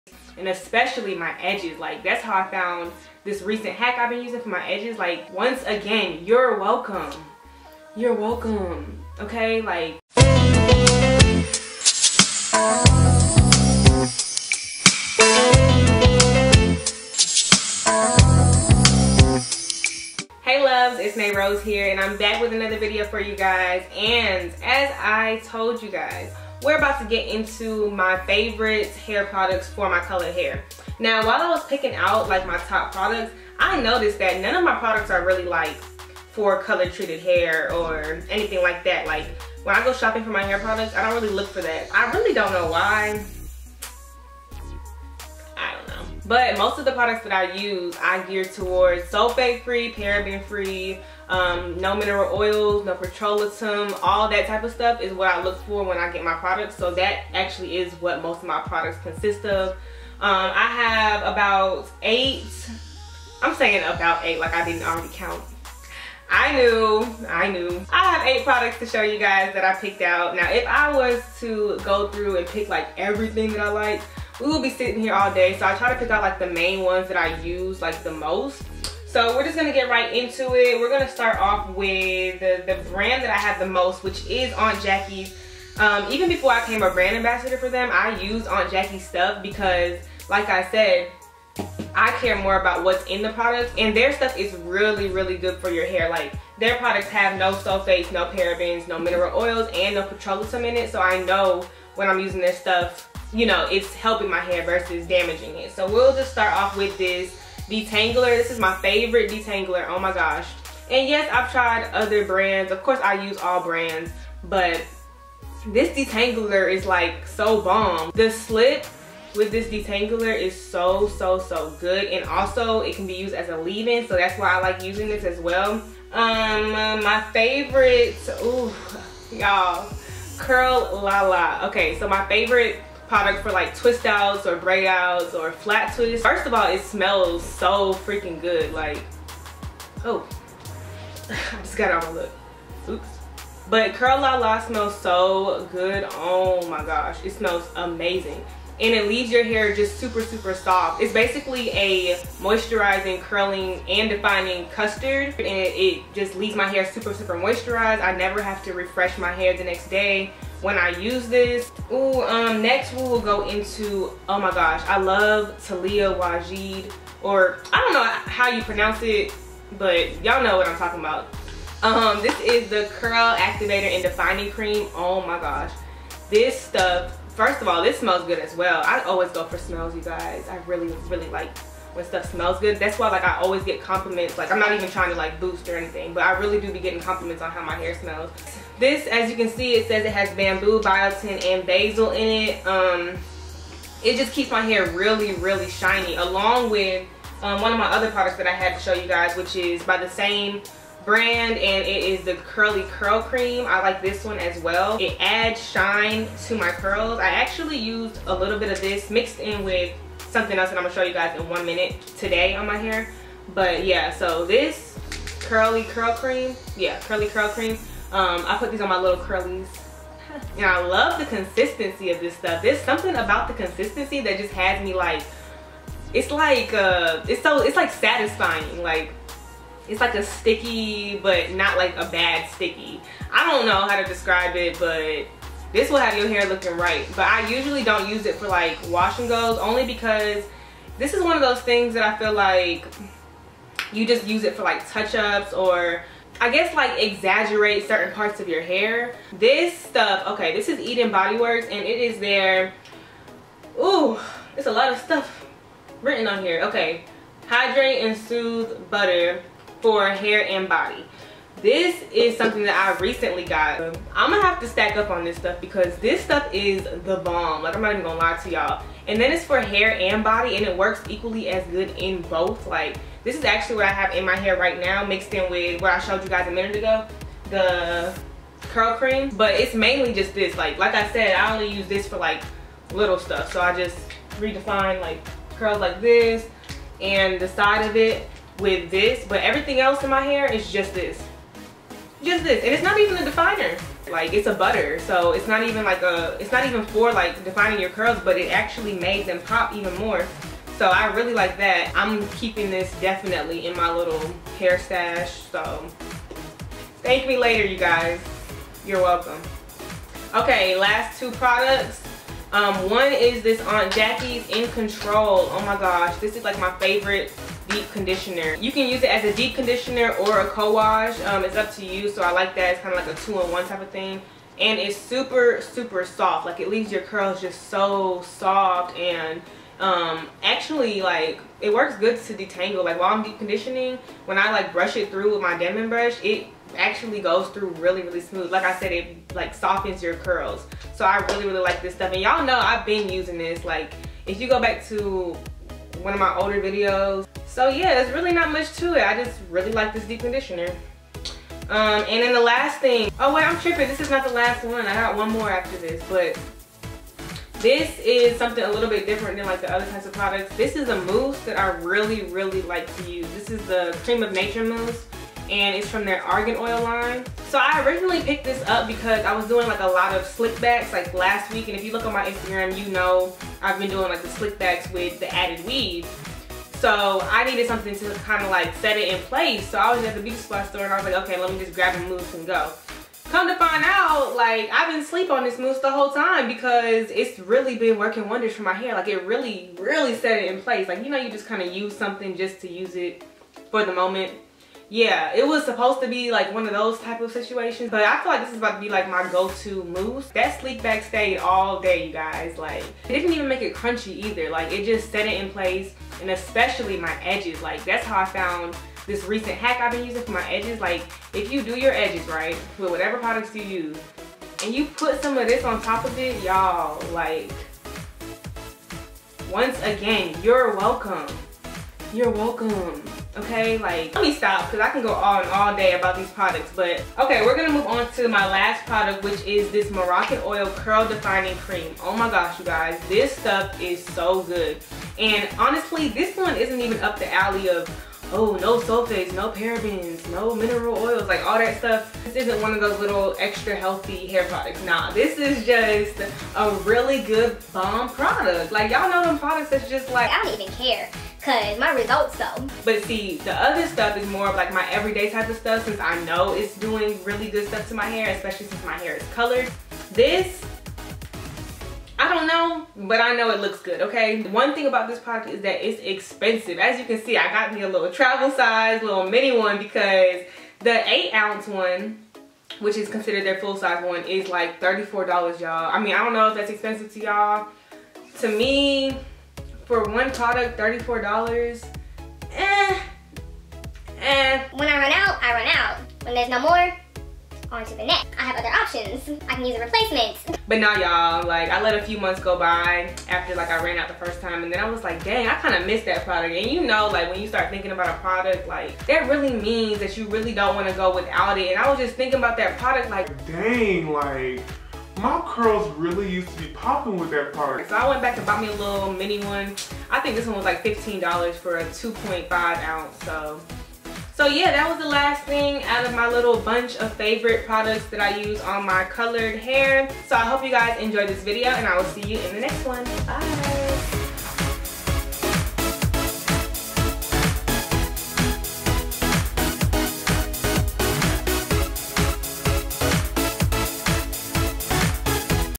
Natural Hair Products Your Colored Hair NEEDS | WORKS WONDERS
Hey Loves ! This video is all about my favorite curly hair products!